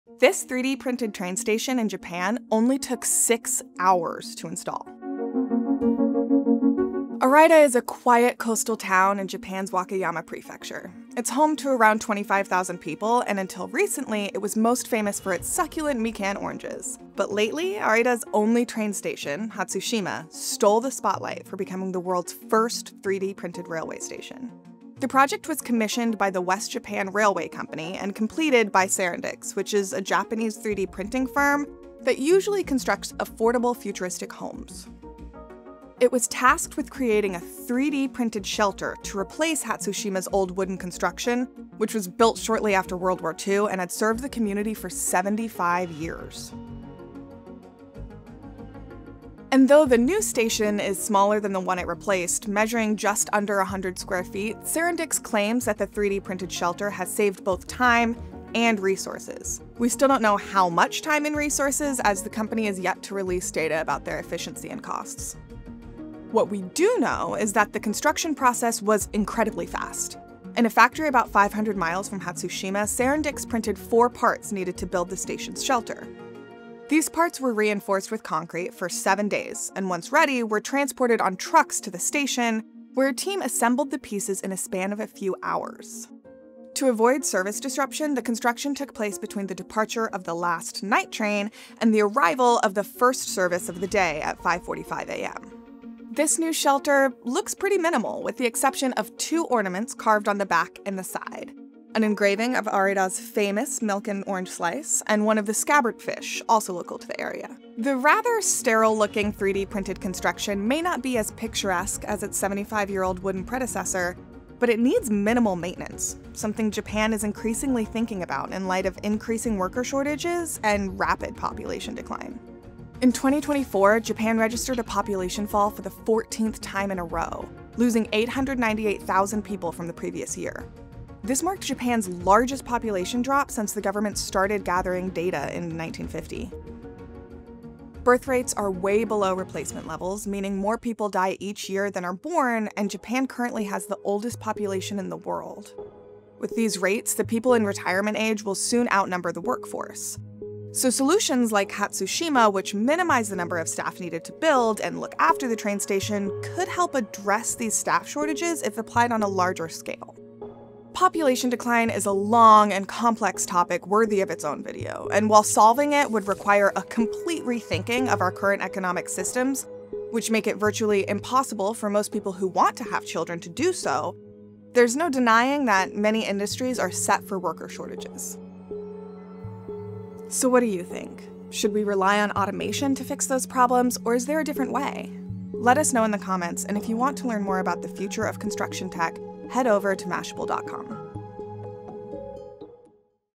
Japan Built the World’s First 3D-Printed Train Station in 6 Hours
A 3D-printed train station, assembled in just a few hours, could be one solution to Japan's worker shortages, driven in part by the country's population decline.

00:00 Introducing the 3-D printer train station
00:49 How the Hatsushima station it was built
1:49 Connection to worker shortages and population decline in Japan